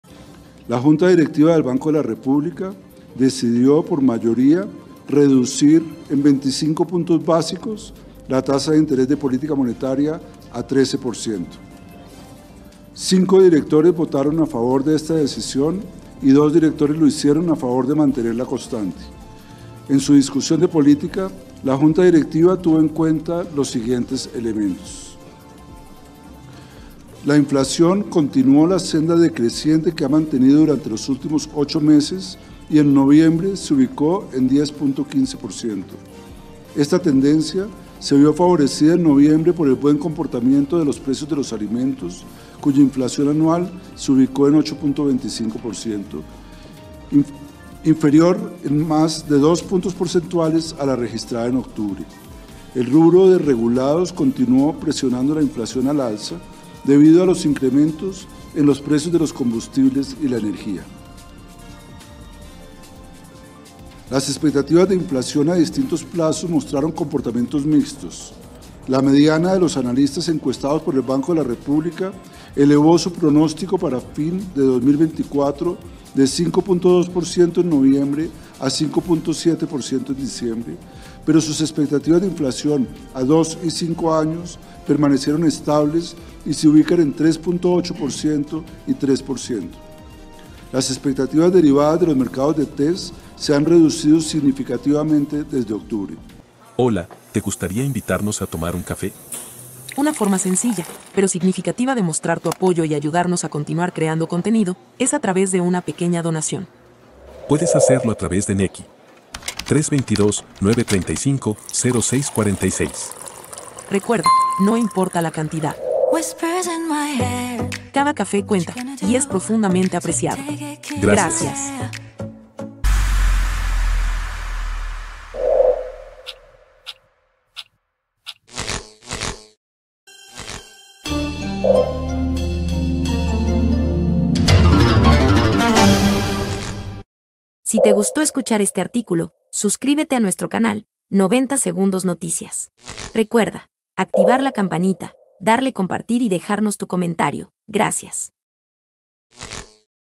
🛑🎥Junta del Banco de la República bajó su tasa de intervención en 0,25 puntos a 13 %👇👇
"Lo importante de esta decisión es el mensaje que se le envía al mercado en una coyuntura difícil del mercado, los bancos y las empresas van a poder contar con mejores tasas para los cierres financieros (de algunos proyectos), y el mensaje también va en el sentido de que la reducción de tasas en el mundo real también es un objetivo", dijo Ricardo Bonilla Ministro de Hacienda, al término de la reunión de  este viernes.👇👇
90 Segundos Noticias, Somos independientes, con neutralidad e imparciales, objetivos y honestos en su concepto estricto. Sin prejuicios, justos y equilibrados. ...

¡Gracias! 🌟🤝